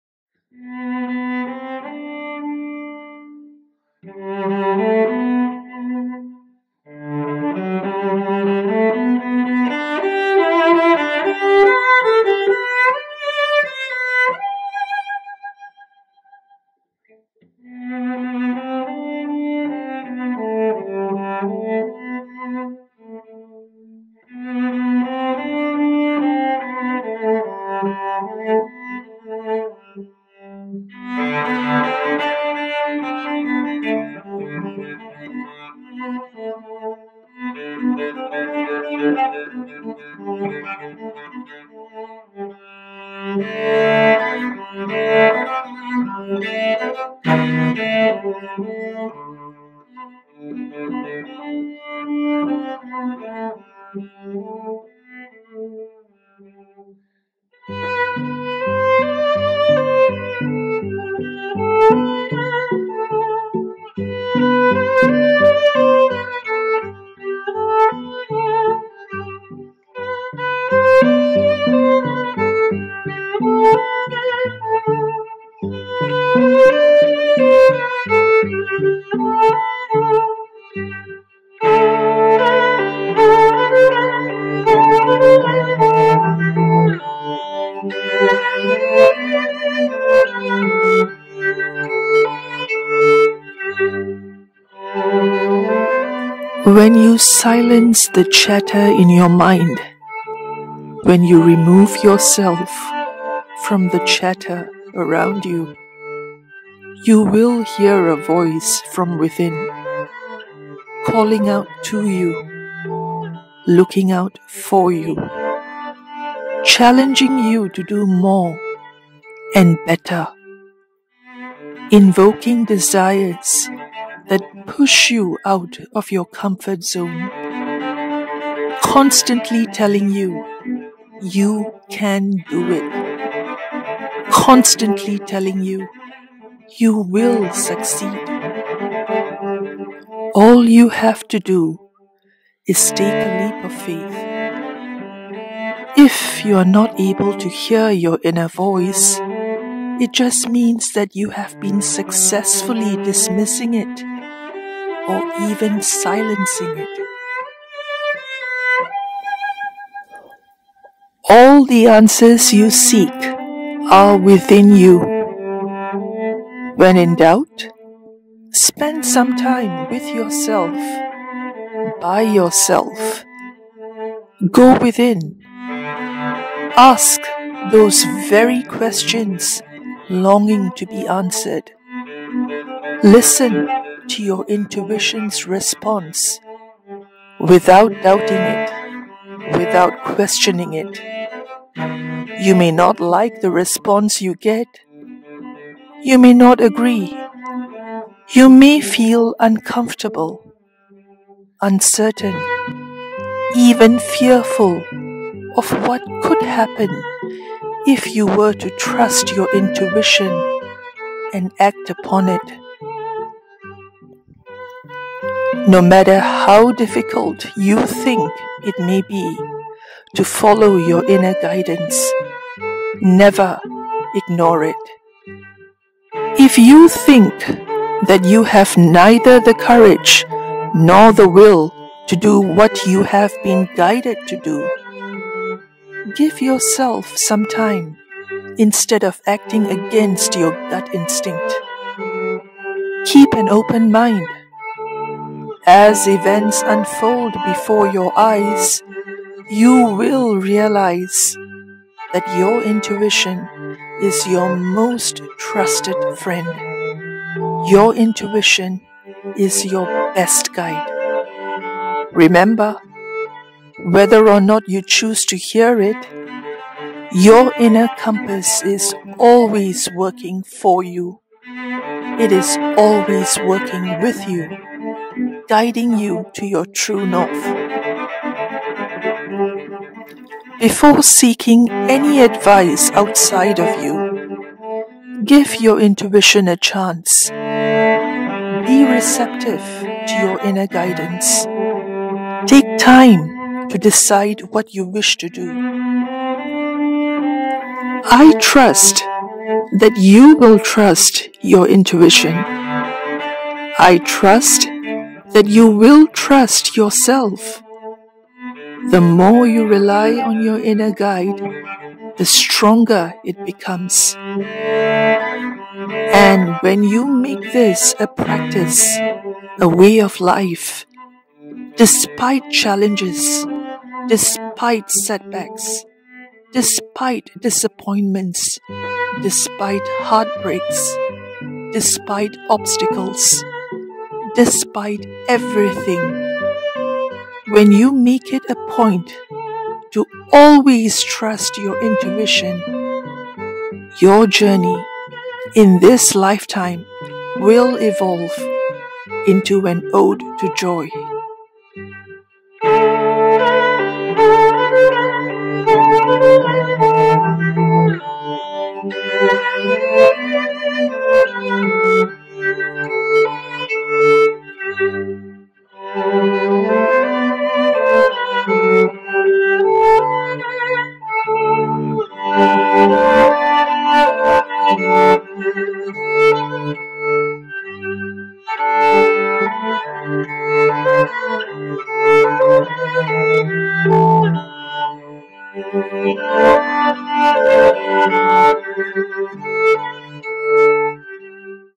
How To Trust Your Intuition
Your intuition is your best guide. It never leads you astray. It leads you to Your Truth, you true desires. It ensures that you are on the path of your life purpose.

So how do you trust your intuition? Have you been trusting your intuition? Or have you been silencing it, in order to live the life you are expected to live? Are you living up to your own expectations? These are some questions for you to ponder.

Be honest with yourself. For, your intuition is always absolutely honest with you. It is your best friend for life! Trusting your intuition would mean trusting yourself. And there is nothing meaningful that you can accomplish on your journey, without trusting yourself.

Choose to trust your inner compass. Allow your inner guide to lead you to your True North. Remember...You Have Free Will!